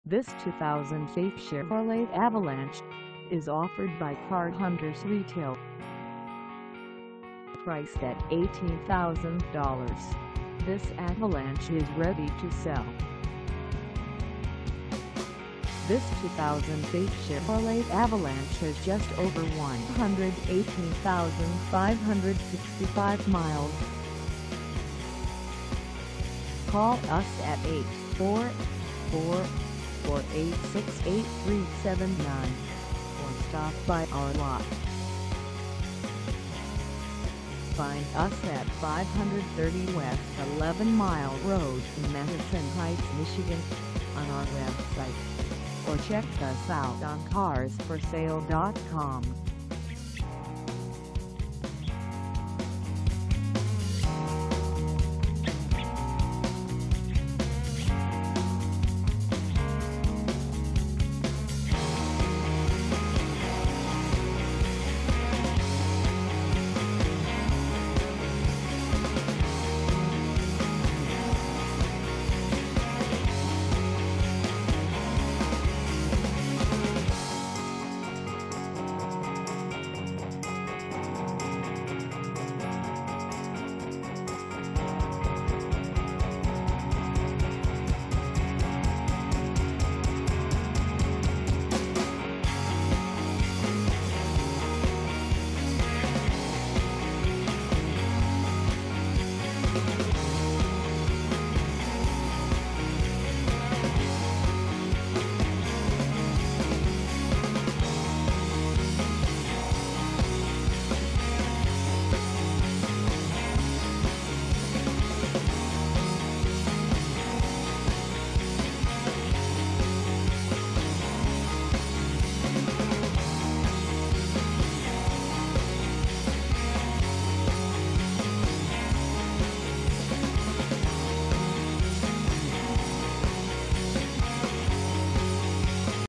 2008 Chevrolet Avalanche LTZ 4x4 4dr Crew Cab SB for sale in
This 2008 Chevrolet Avalanche LTZ 4x4 4dr Crew Cab SB is for sale in Madison Heights, MI 48071 at Car Hunterz Retail.

Engine: 5.3L V8
Transmission: Automatic 4-Speed
Color: Pewter
Mileage: 118565
Contact Car Hunterz Retail 530 West Eleven Mile Rd Madison Heights, MI 48071 to test drive this 2008 Chevrolet Avalanche LTZ 4x4 4dr Crew Cab SB today.